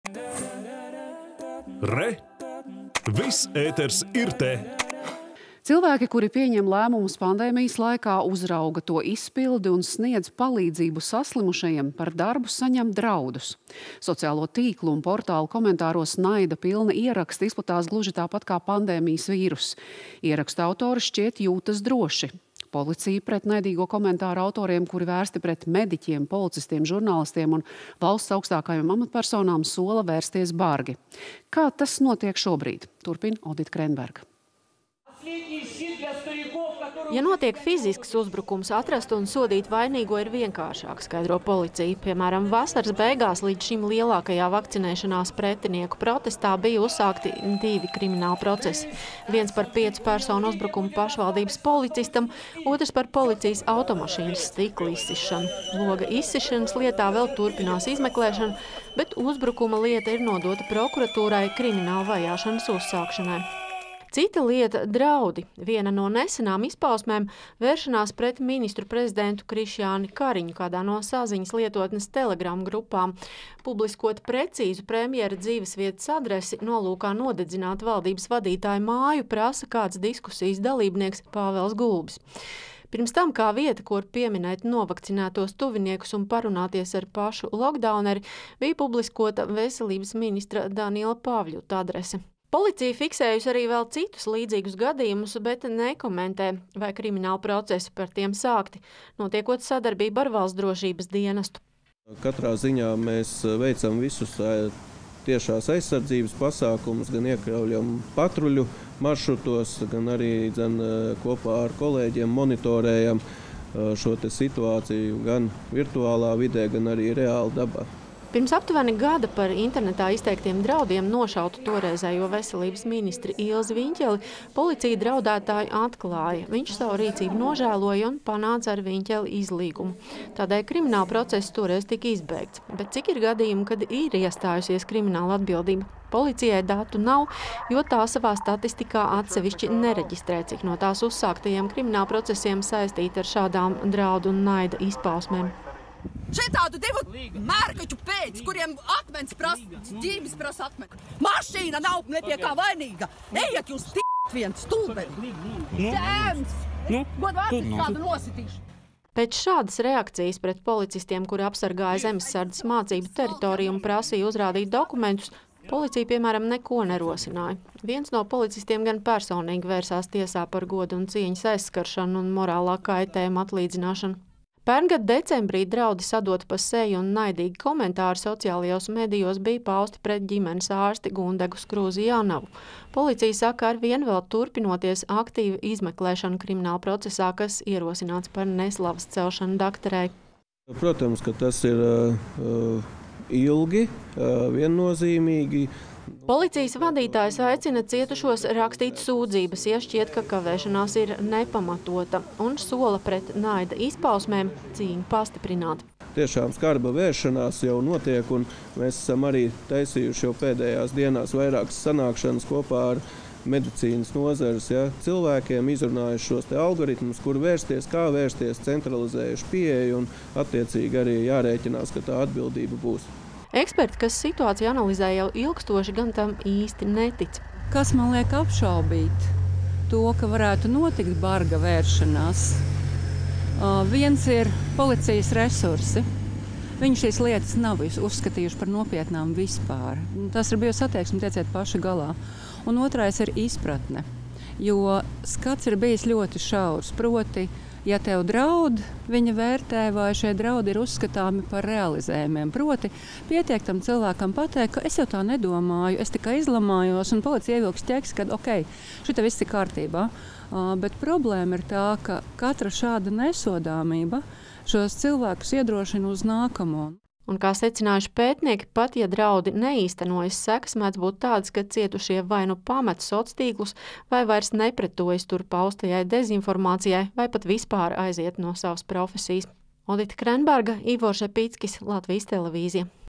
Policija sola bargi vērsties pret naida un draudu paudējiem
Cilvēki, kuri pieņem lēmumus pandēmijas laikā, uzrauga to izpildi un sniedz palīdzību saslimušajiem par savu darbu saņem draudus.
Sociālo tīklu un portālu komentāros naida pilni ieraksti izplatās gluži tāpat kā pandēmijas vīruss. Ierakstu autori šķiet jūtas droši. Policija pret naidīgo komentāru autoriem, kuri vērsti pret mediķiem, policistiem, žurnālistiem un valsts austākajām amatpersonām, sola vērsties bargi. Kā šobrīd tas notiek? Turpina Odita Krenberga.